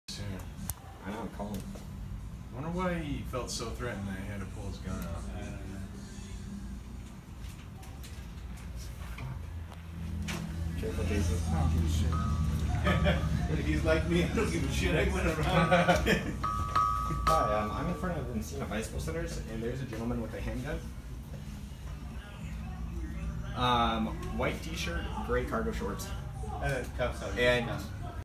Off-duty Vallejo police officer pulls gun on man in Walnut Creek
Vallejo off duty officer David McLaughlin is seen pointing gun at a man during an altercation at the Encina Grande shopping center in Walnut Creek on August 11, 2018